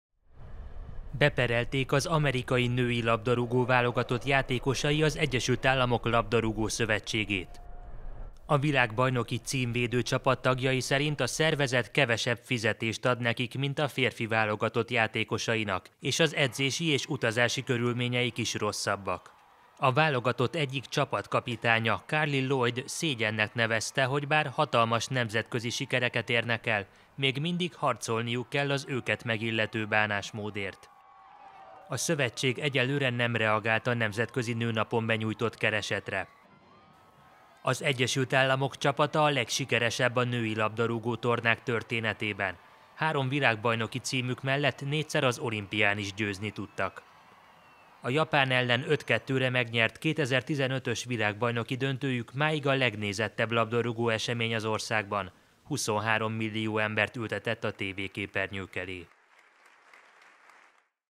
Egyenlő bánásmódot követlenek a női focisták az Egyesült Államokban
A világbajnoki címvédő válogatott tagjai beperelték a szövetséget, mert az jobb fizetést ad a férfi játékosoknak.…